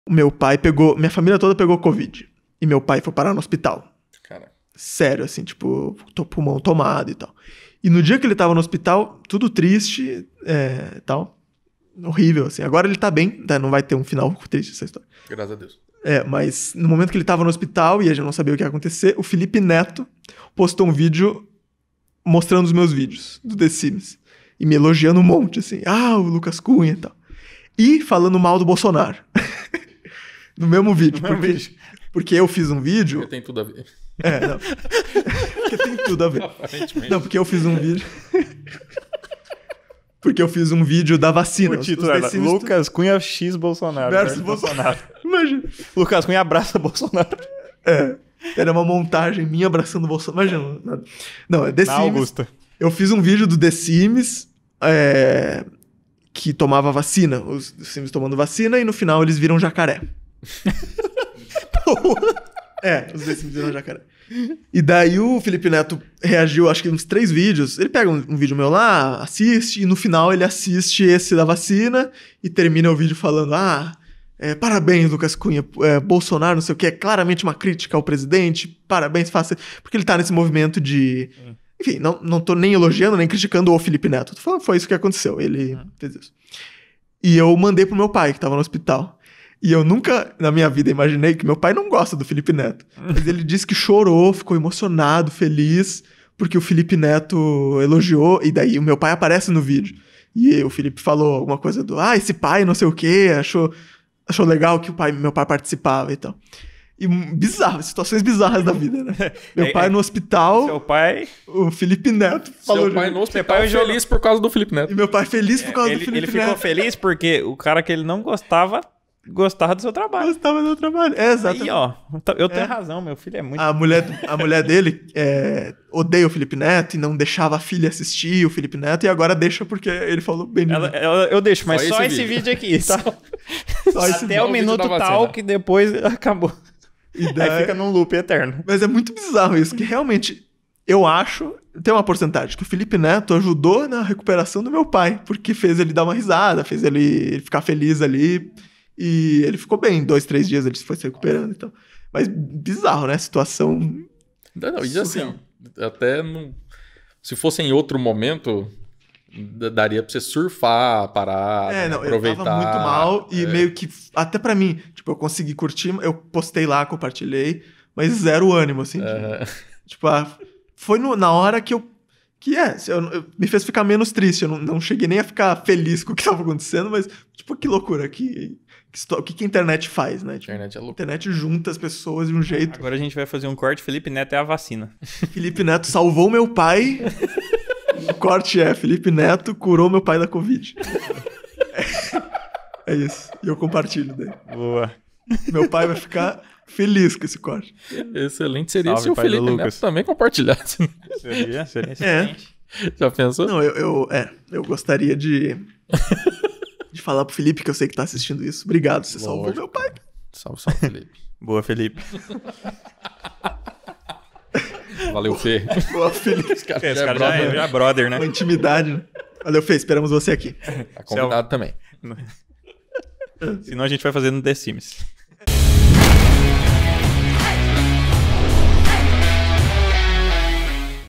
FELIPE NETO AJUDA PAI DE LUCAS CUNHA EM MOMENTO CRÍTICO
Lucas Cunha conta sobre quando a família toda dele pegou C*V1D e o pai dele ficou mal com o pulmão debilitado e ficou internado no hospital. Nesse dia Felipe Neto postou um vídeo reagindo a um vídeo do Lucas Cunhas que ele tinha feito em relação ao The Sims em que os personagens tomavam a vacina e viravam jacaré. Felipe Neto entendeu que o vídeo era um crítica ao Bolsonaro e compartilhou o vídeo também pela participação do pai e achou isso legal. Esse foi um dos motivos que ajudou a recuperação do pai do Lucas Cunha.